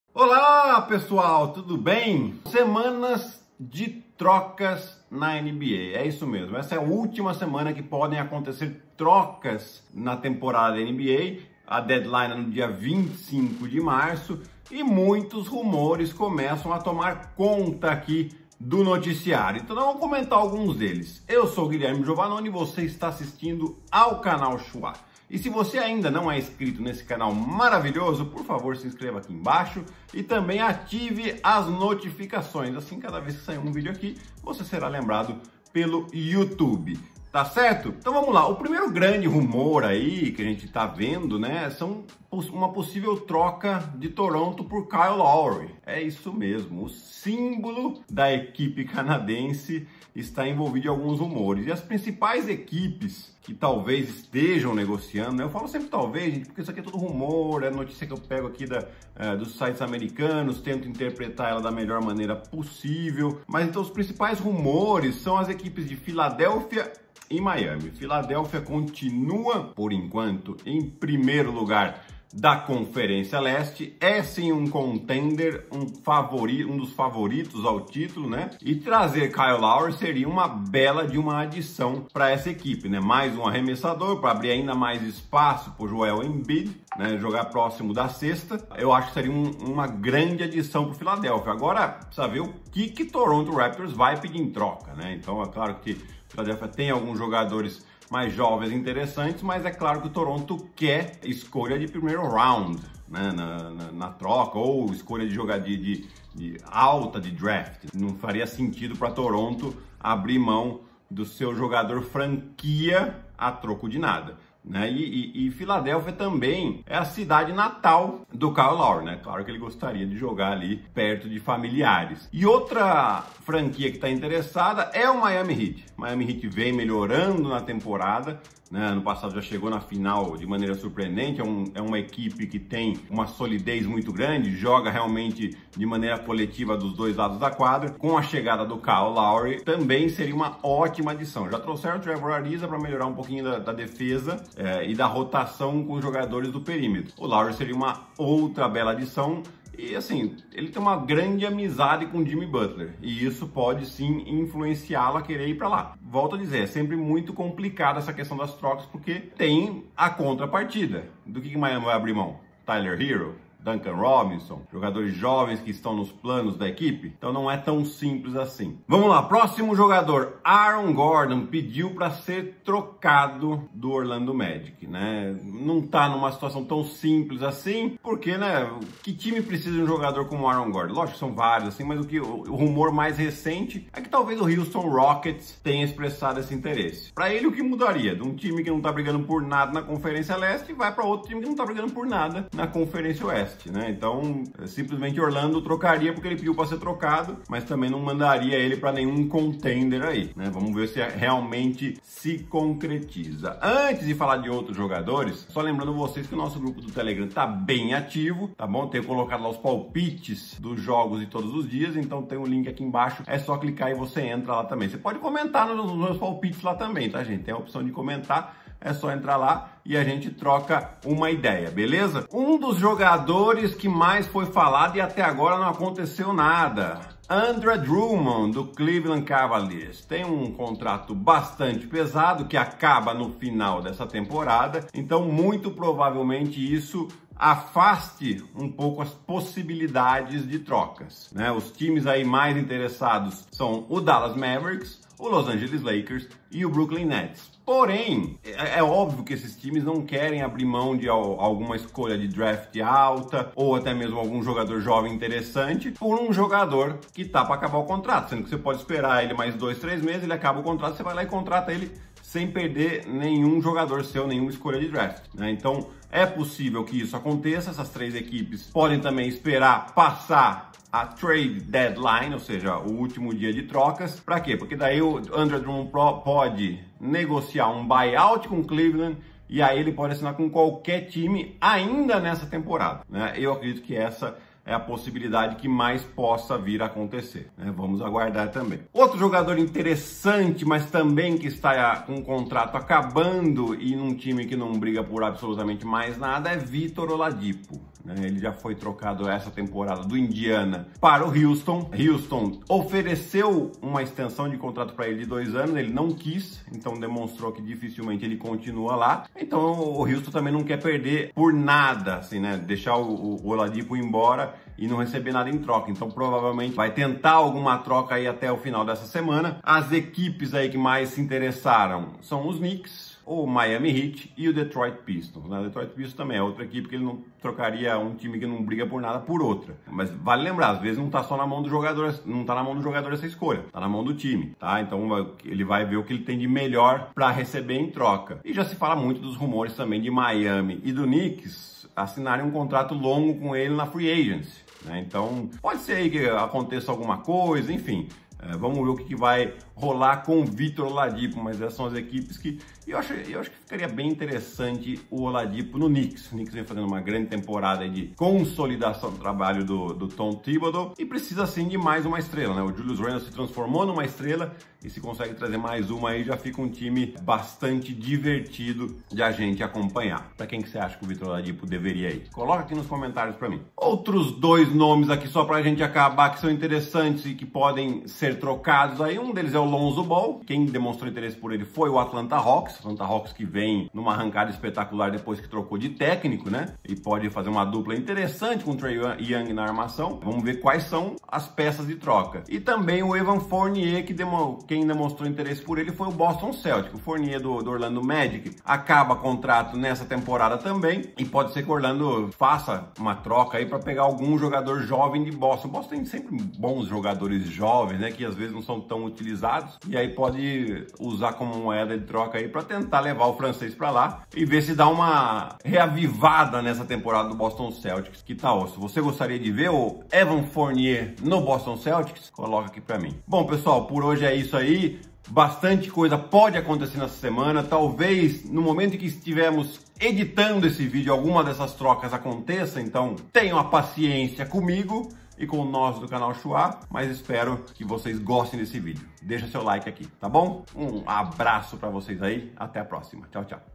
Kyle Lowry fora do Toronto Raptors? - ÚLTIMAS TROCAS - Análise do Giovannoni
Última semana de trocas na NBA e temos muitos rumores. Kyle Lowry desperta interesse do Philadelphia 76ers e de Miami Heat.
Victor Oladipo não aceitou a proposta de renovação do Houston Rockets e pode ser trocado. New York Knicks, Miami Heat e Detroit Pistons tem interesse no jogador.
E André Drumond ainda não tem seu futuro definido. Los Angeles Lakers, Brooklyn Nets e Dallas Mavs são os possíveis destinos. Pra onde ele vai?